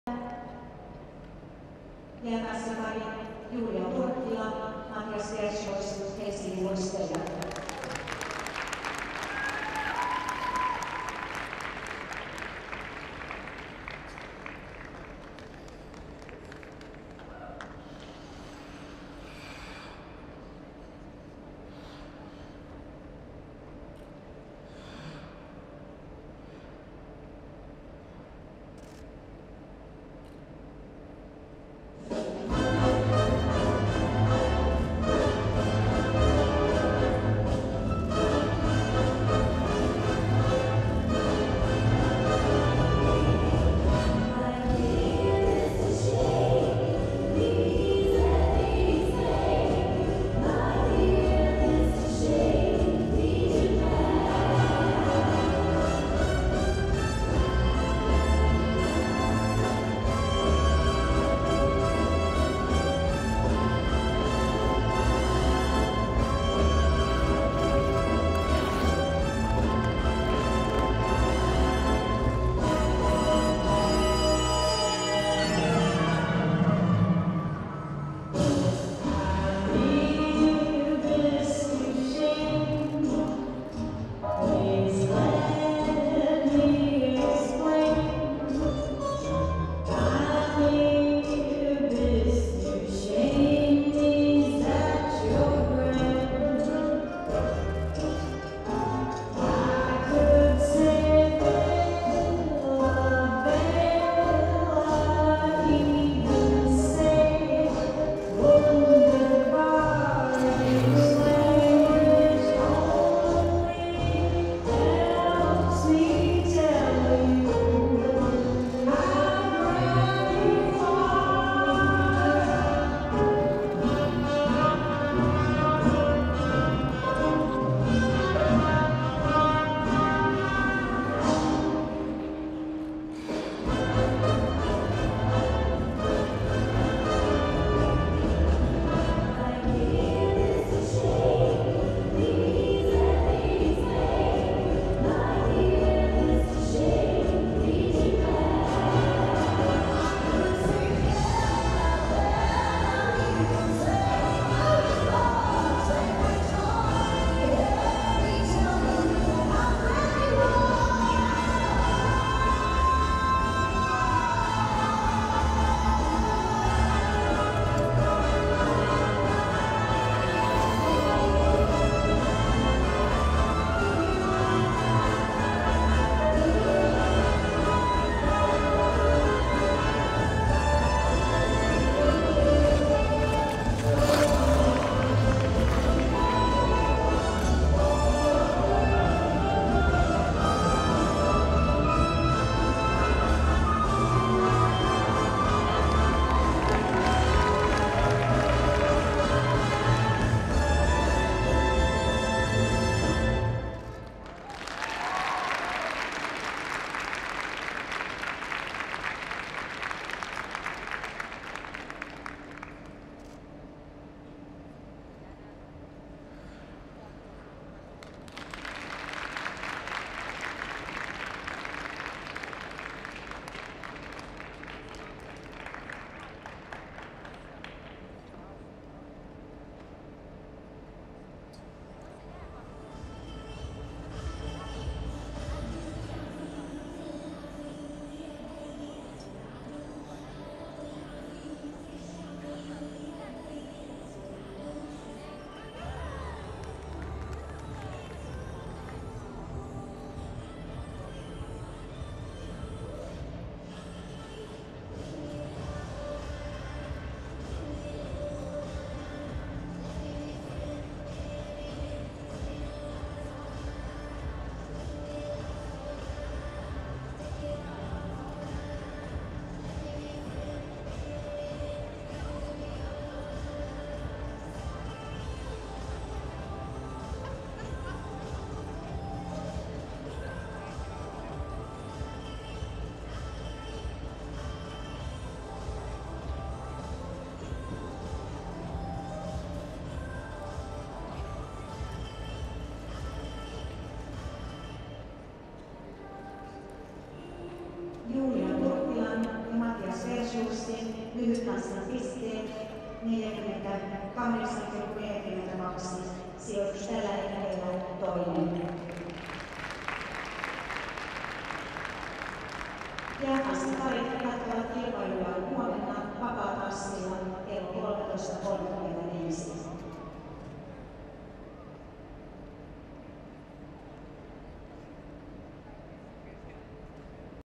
Figure Skating Ice Dance Short Dance Seniors Juulia Turkkila / Matthias Versluis
They are new ice dance senior pair this season from Finland. I think this couple is a good match and very athletic. Juulia Turkkila is glowing!